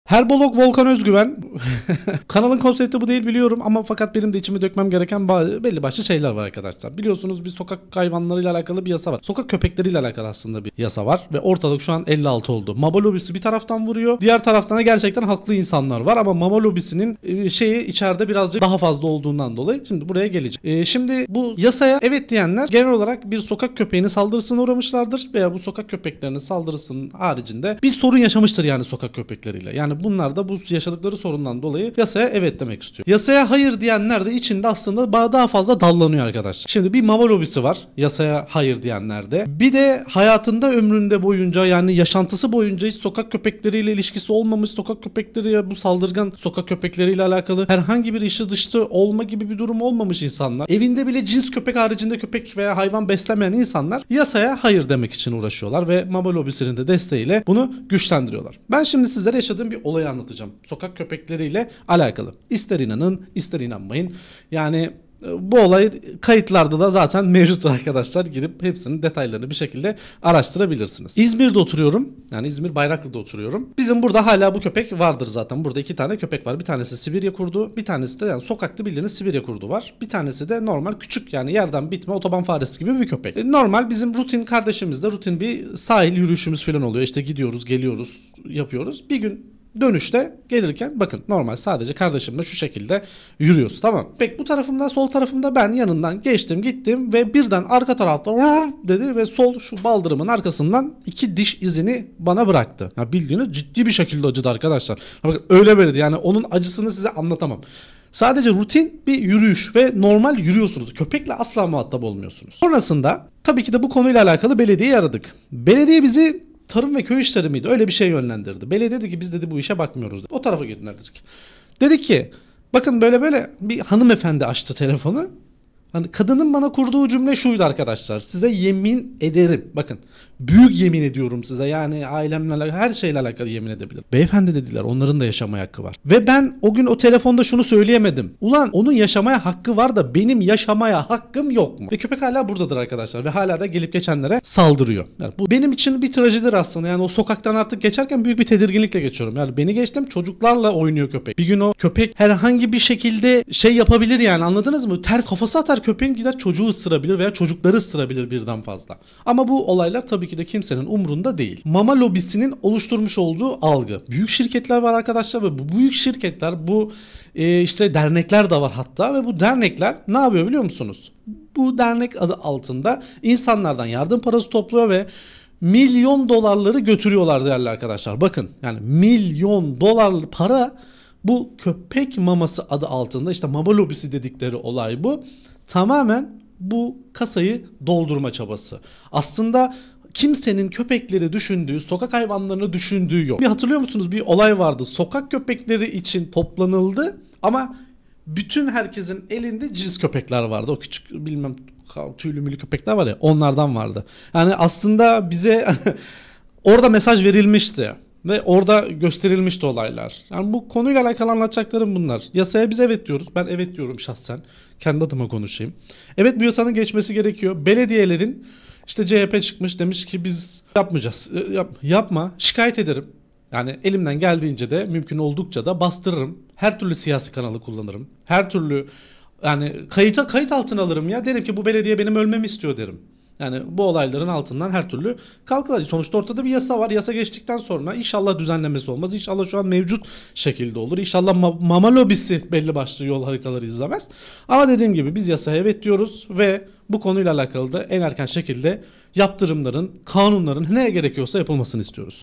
Sokak Köpekleri Sorunu: Mama Lobisi Gerçekleri ve Yaşadığım Saldırı!
Sokak köpekleri ile ilgili büyük bir sorunu ele alıyoruz. Mama lobisinin, sokak hayvanları konusunda neden hiçbir önlem alınmadığını nasıl etkilediğini anlattım. Bu videoda, sokak köpeklerinin saldırısına uğradığım kendi deneyimimi paylaşıyor ve bu konuda neden acil önlem alınması gerektiğini tartışıyoruz. Sokak köpekleri sorununu ve mama lobisinin etkilerini daha yakından incelemek için videoyu izleyin!

🔴 Bu videoda:

Mama lobisinin etkisi ve gerçekler
Sokak köpeği saldırısına uğradığım an
Sokak hayvanları konusunda alınması gereken önlemler
Kendi deneyimlerim ve çözüm önerilerim
📢 Görüşlerinizi ve deneyimlerinizi yorumlarda paylaşmayı unutmayın!